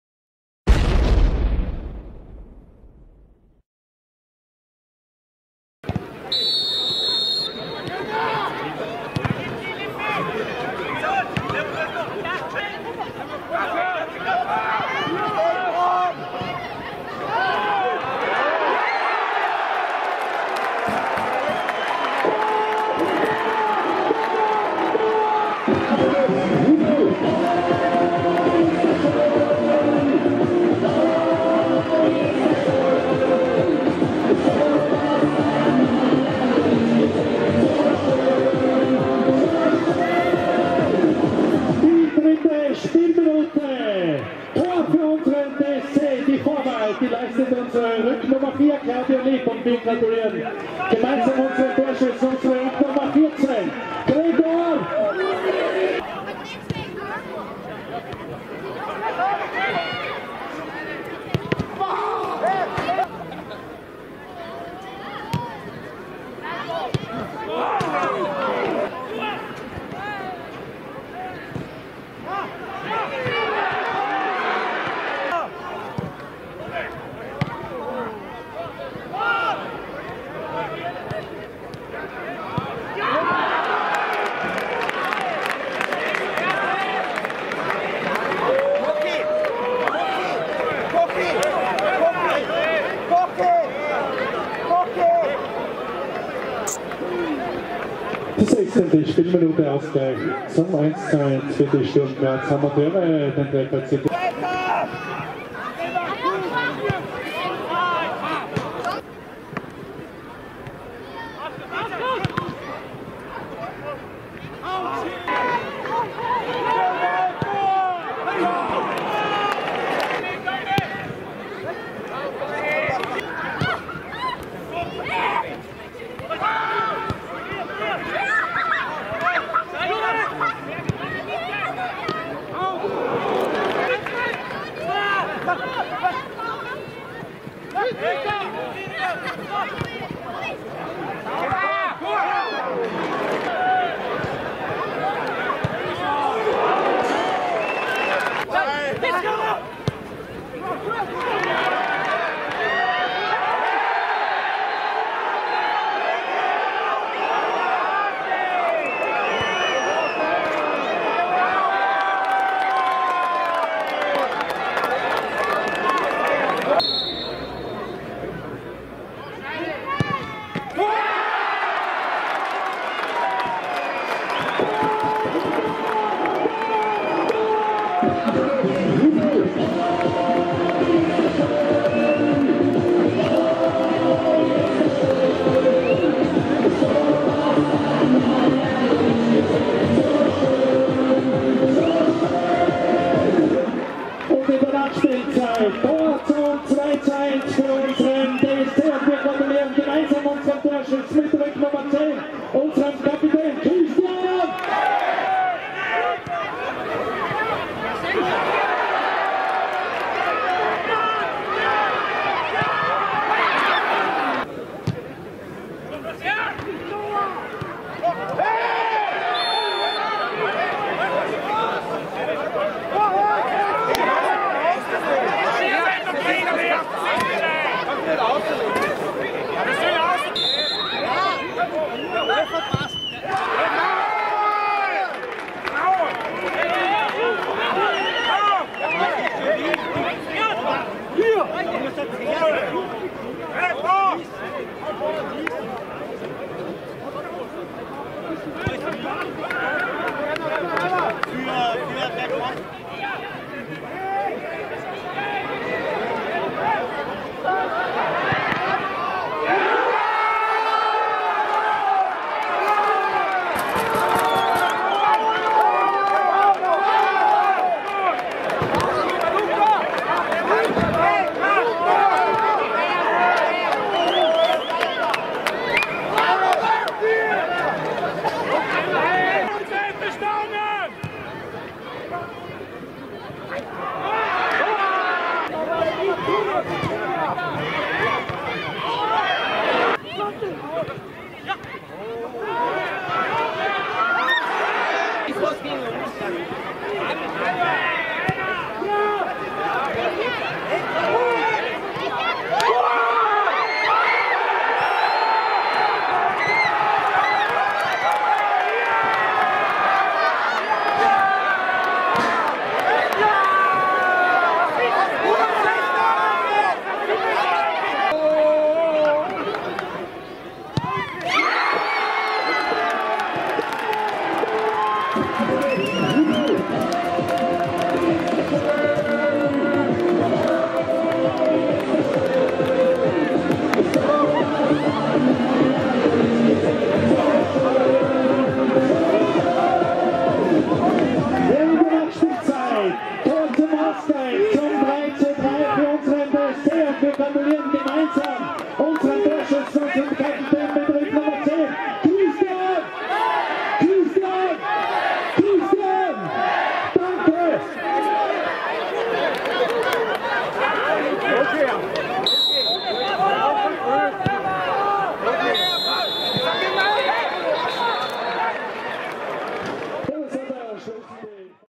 2019 03 29 DSC Strum Graz Amateure 3:3 (2:1)
19. Runde Regionalliga Mitte 2018/19
DSC Wonisch Installationen : Sturm Graz Amateure 3:3 (2:1)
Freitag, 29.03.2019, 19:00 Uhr
Koralmstadion - 700 Zuseher